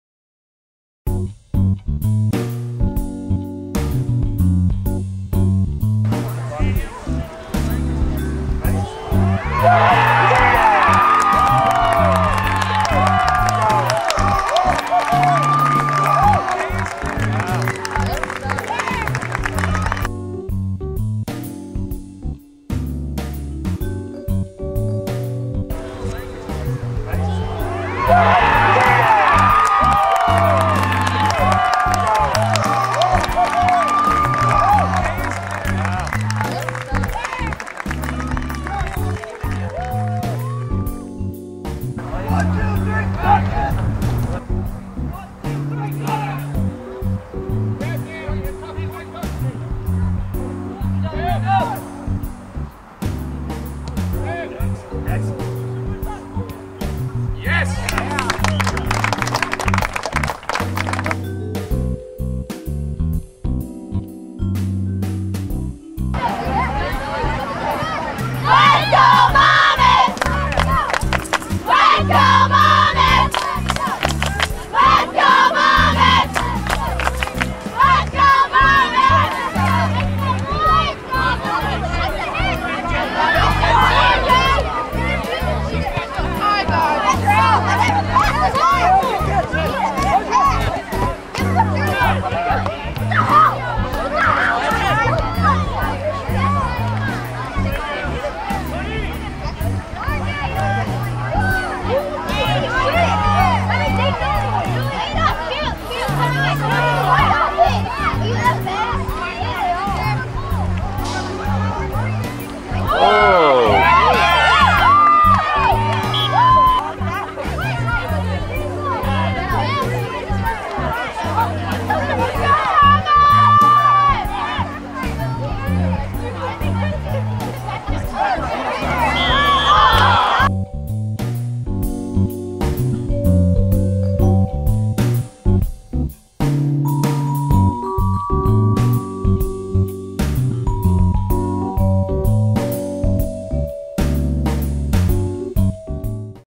SportsVideographybyTR
Excerpts from High School, Soccer Academy (NJSA04) and College Soccer Games